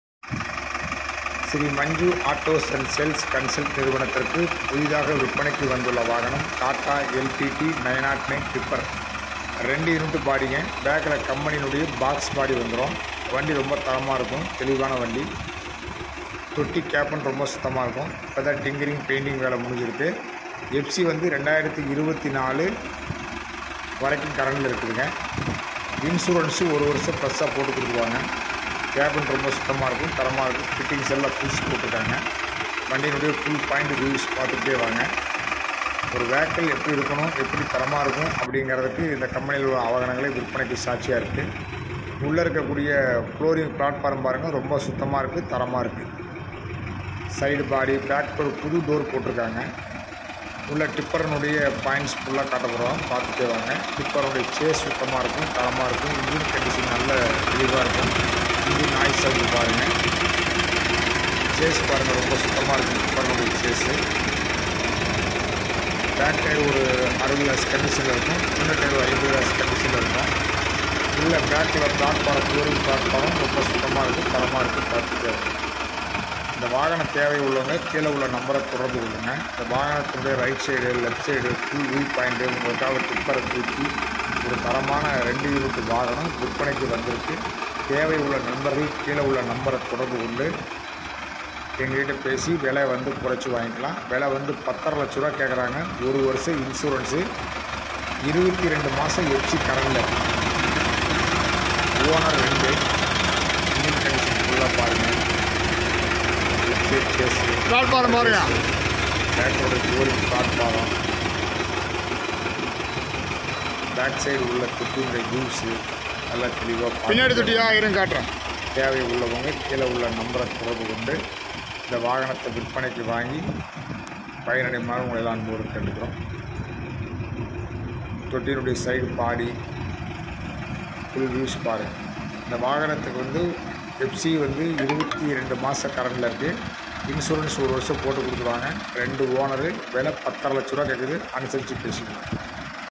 FORSALE TATA MOTORS LPT
MAKE : TATA  MOTORS  909 TIPPER
REGISTRATION : TN : 88
MODEL : 2016
FITNESS : 23/08/2024
INSURANCE : 1 YEAR
TAX : 30/06/2024
OWNER : 2
KM : 98660
TYER : 6 ORIGINAL 50 %
ENGINE TYPE BS : 3
ARI BREAK
POWER STEARING
BODY : TIPPER
UINT : 2 UINT
PASSING : 10400 (GVW)
PRICE : 1050000
LOAN APPROVAL 80 %
LOCATION : NAMAKKAL